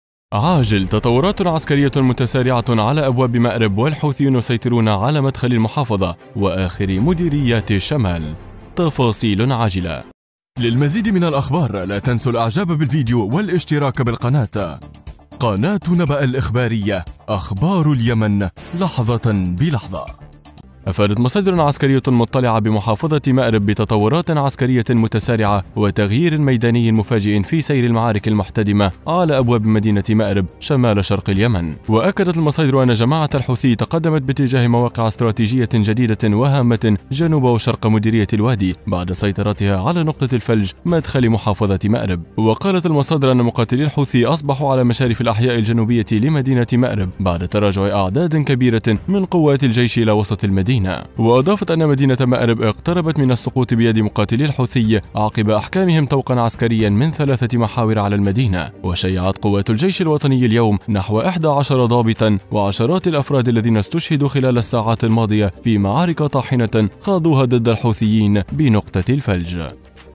عاجل الحوثيين يسيطرون على مدخل محافظه مارب وآخر مديريات الشمال وهذا ماحدث !!
افادت مصادر عسكرية مطلعة بمحافظة مأرب بتطورات عسكرية متسارعة وتغير ميداني مفاجئ في سير المعارك المحتدمة على ابواب مدينة مأرب شمال شرق اليمن.
واكدت المصادر أن جماعة الحوثي تقدمت باتجاه مواقع استراتيجية جديدة وهامة جنوب وشرق مديرية الوادي بعد سيطرتها على نقطة الفلج مدخل محافظة مأرب .
وقالت المصادر أن مقاتلي الحوثي أصبحوا على مشارف الأحياء الجنوبية لمدينة مأرب بعد تراجع أعداد كبيرة من قوات الجيش إلى وسط المدينة.
وأضافت ان مدينة مأرب اقتربت من السقوط بيد مقاتلي الحوثي عقب إحكامهم طوقا عسكريا من ثلاثة محاور على المدينة .
و شيعت قوات الجيش الوطني اليوم  نحو 11 ضابطا وعشرات الافراد الذين استشهدوا خلال الساعات الماضية في معارك طاحنة خاضوها ضد ” الحوثيين ” بنقطة الفلج.